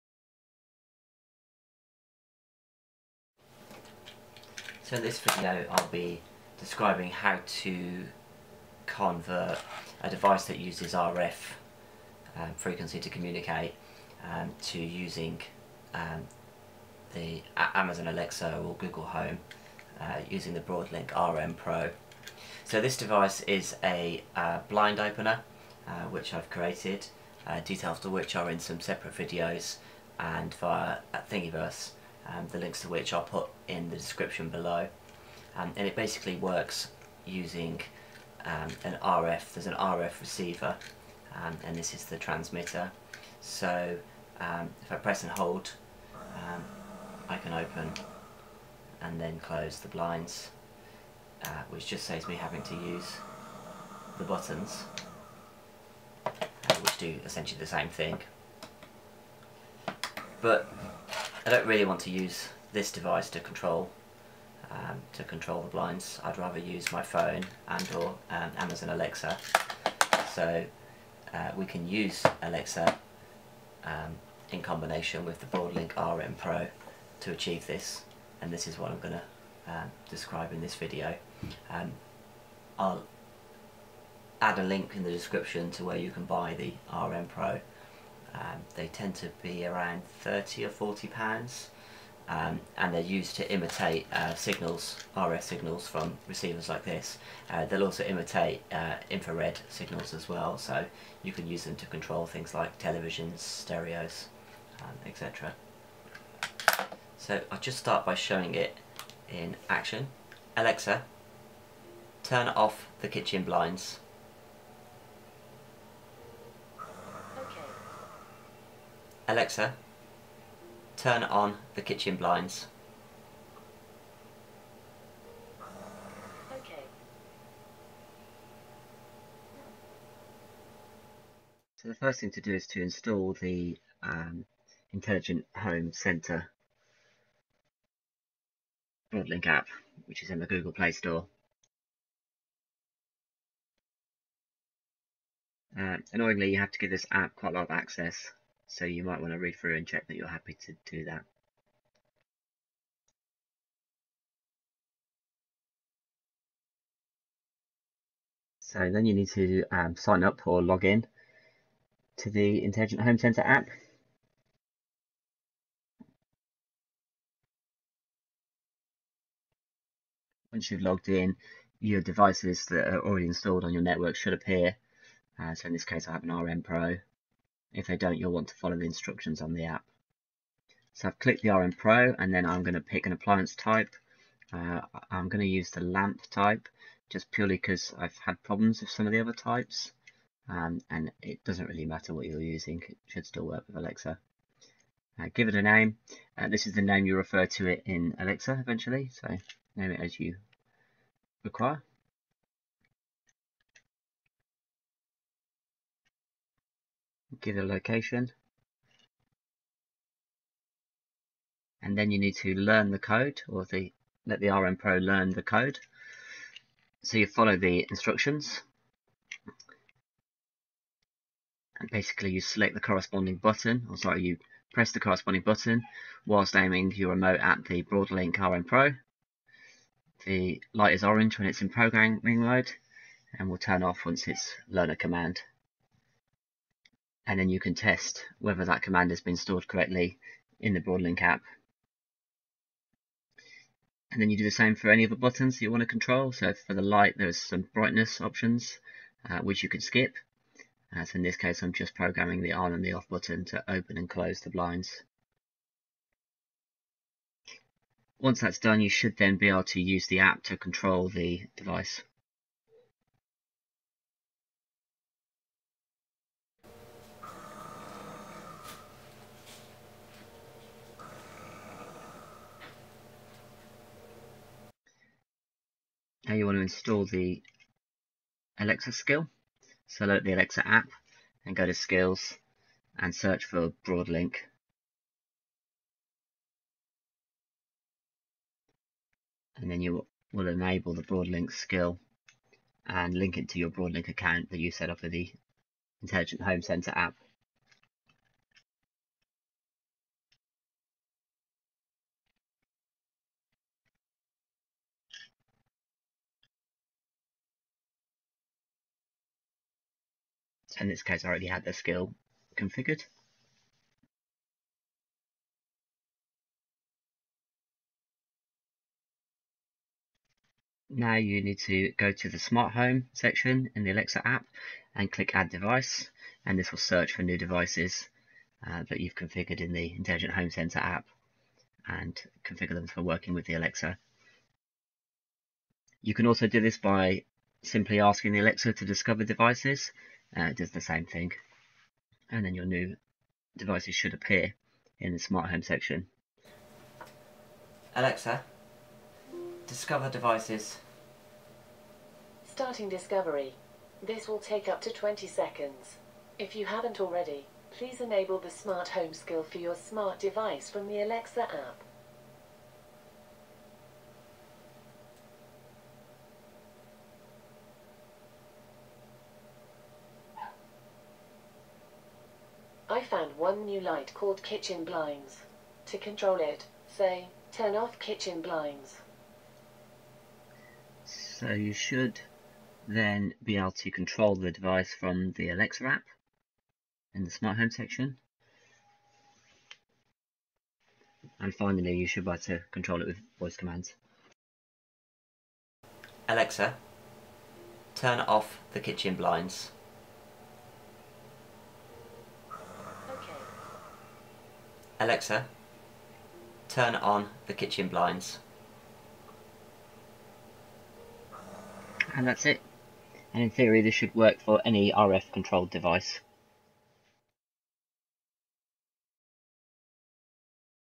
How to configure Amazon Echo to control RF devices using the Broadlink RM Pro
Cheap Radio-Frequeny (RF) devices may be easily controlled using Amazon Echo and the Broadlink RM Pro. This short tutorial demonstrates how to configure new devices in the Intelligent Home Centre App and enable the Broadlink skill in the Amazon Echo app to control them.